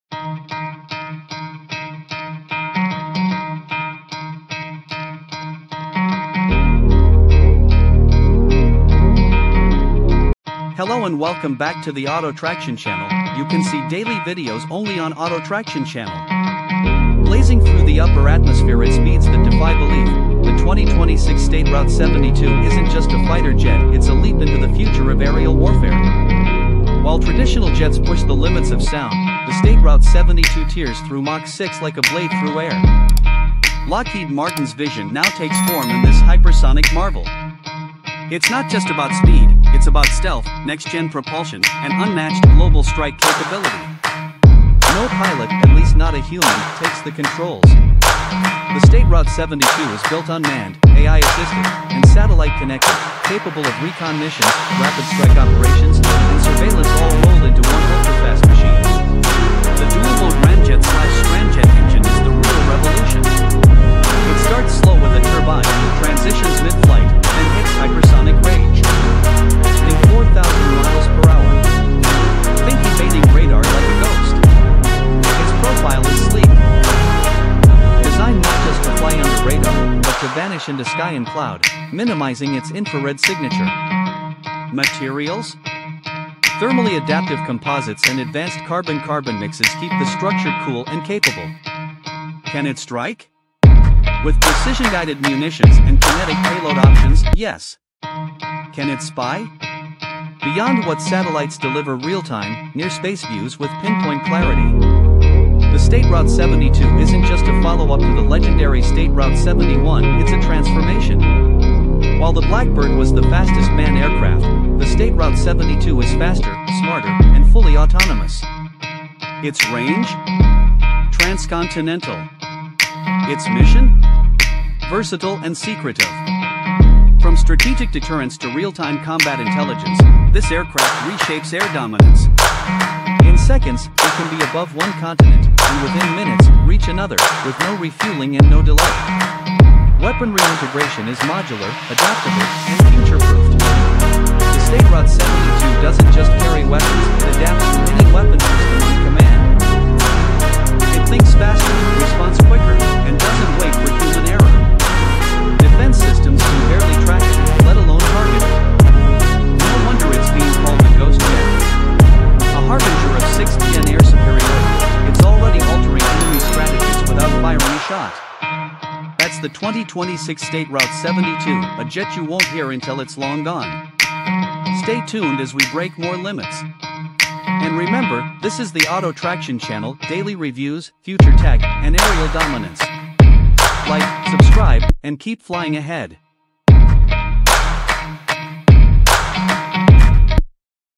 2026 SR-72 Hypersonic Jet Review – The Fastest & Smartest Fighter Ever Built!
2026 SR-72 Hypersonic Jet Review – The Fastest & Smartest Fighter Ever Built!

Discover the power and innovation behind the 2026 SR-72 Hypersonic Fighter Jet in this exclusive review. From unmatched speeds to next-gen stealth and AI-driven capabilities, the SR-72 is reshaping the future of air dominance. Don’t miss the detailed breakdown of performance, technology, and tactical superiority only on Auto Traction Channel.